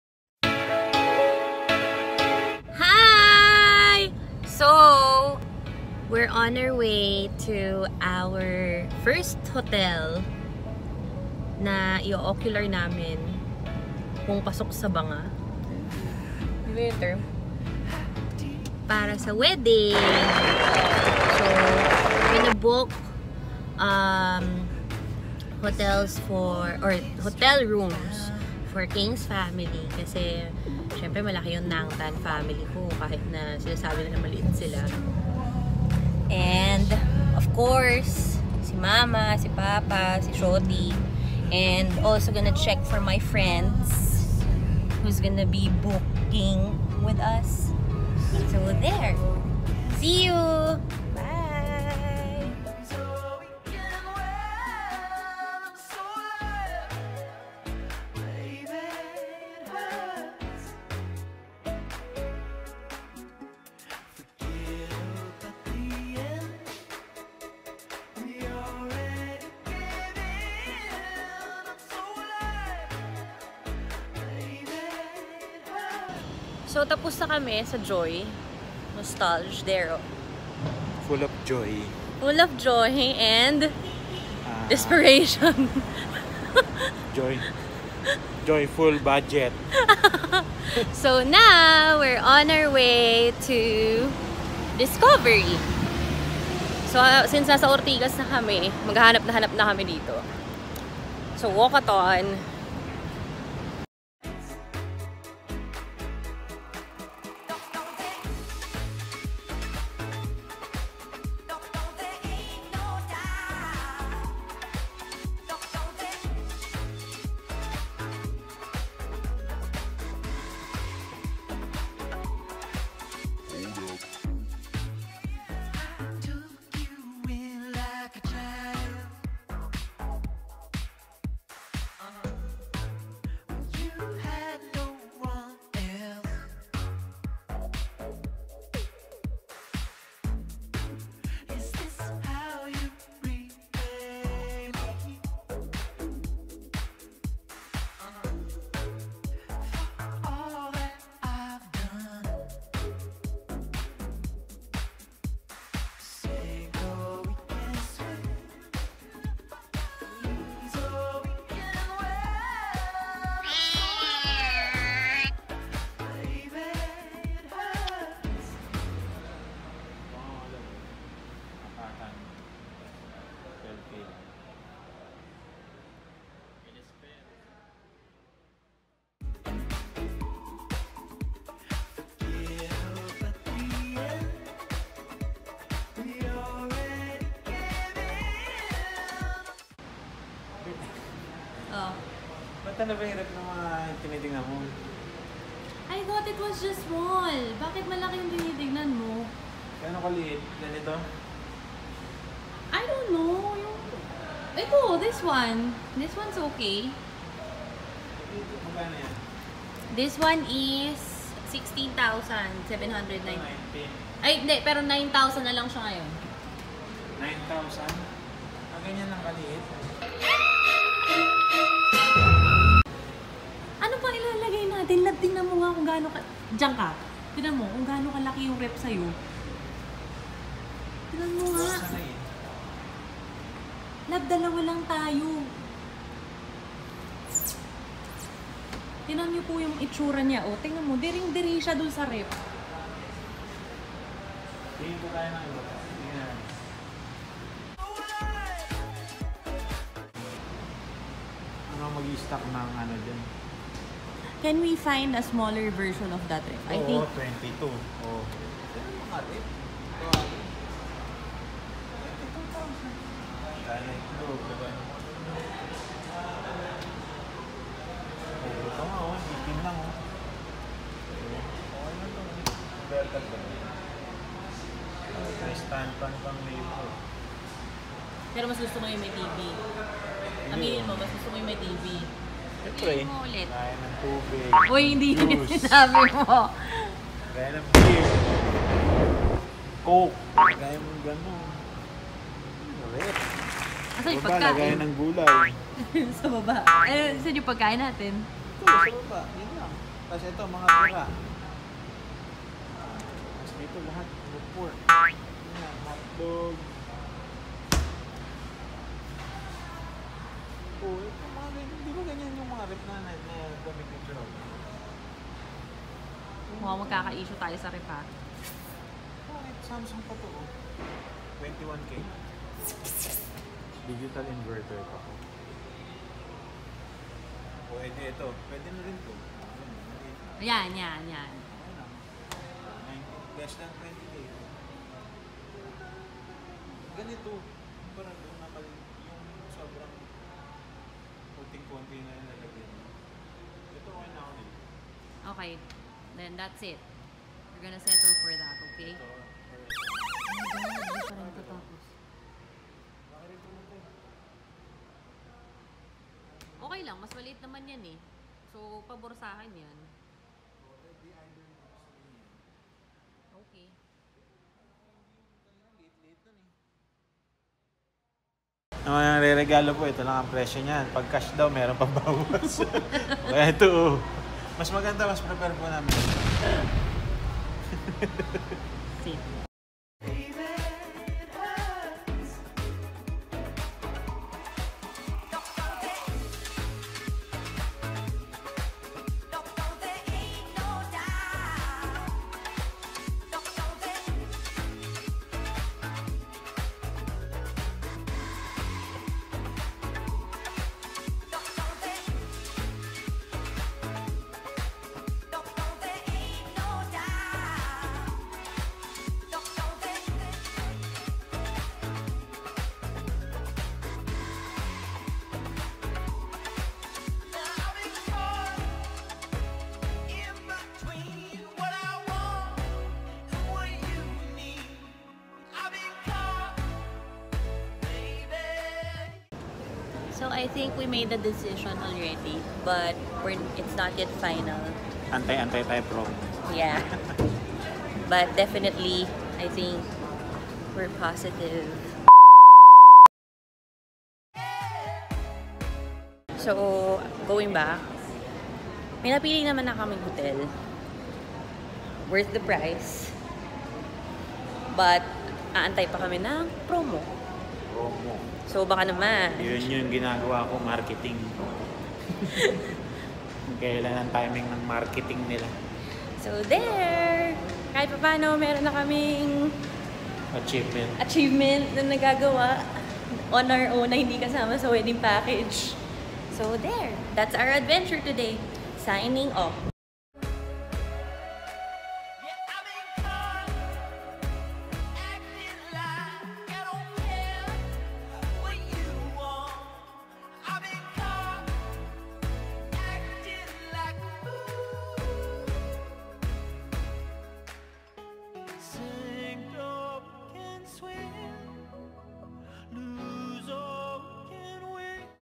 Wedding Vlog#1: HOTEL HUNTING in Ortigas
I told King That i wanted to vlog our road to our wedding! So here’s our first one!!!

Watch our first vlog together for our wedding planning- first stop: what hotel!!!

Sharing you our adventure in search through Ortigas here in manila for hotels- finding the best one that is worth its price and is within our budget. Enjoy!

Plus- we did a random window shopping for our home- watch out on What i discovered would potentially be my arguement with my Husband 😱😱😱